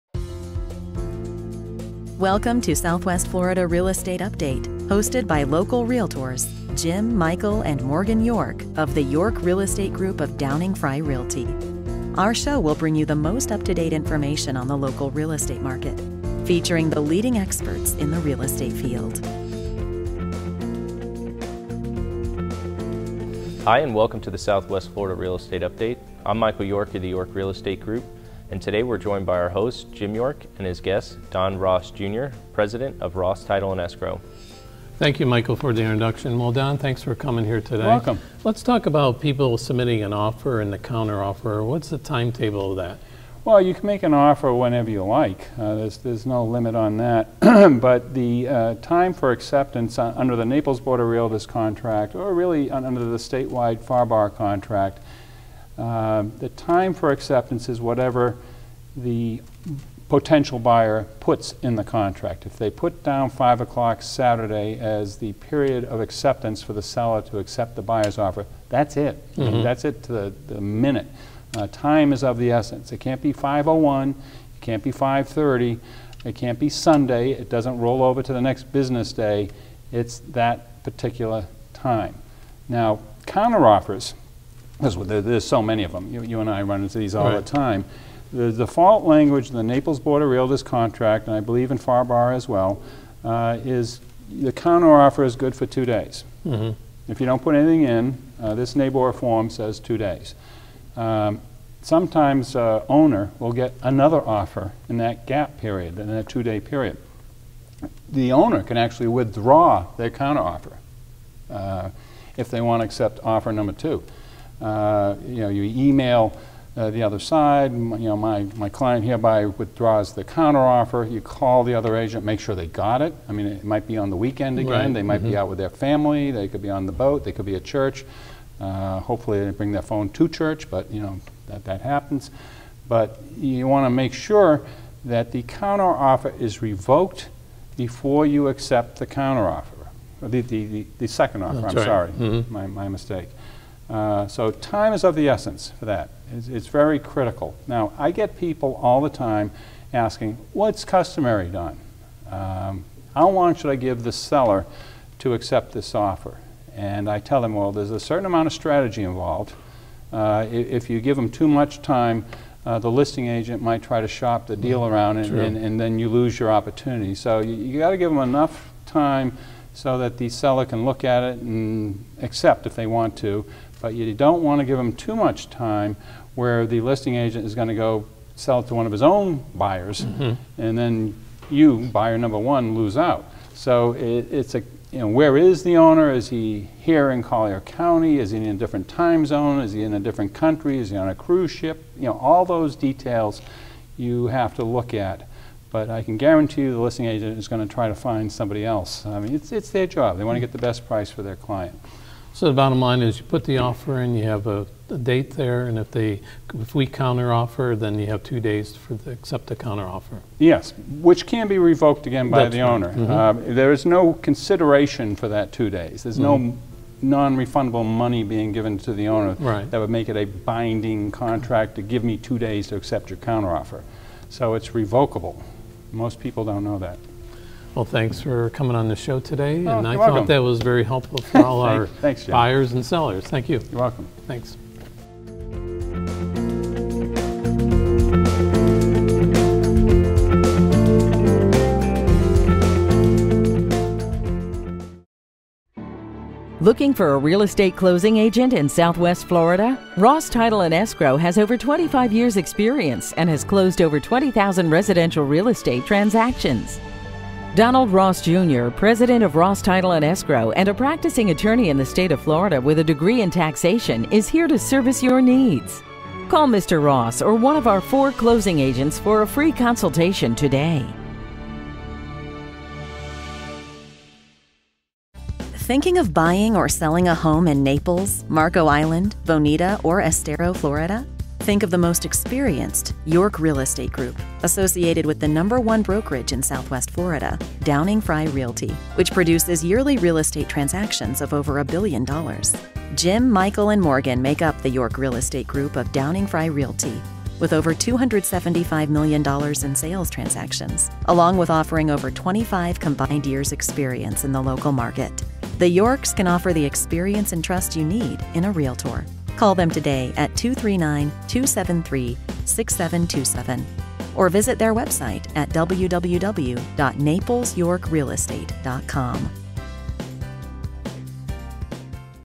SWFL Real Estate Update "Handling Counter Offers"
York Real Estate Group Interviews Attorney Donald Ross Jr, President of Ross Title and Closing ,Naples Fl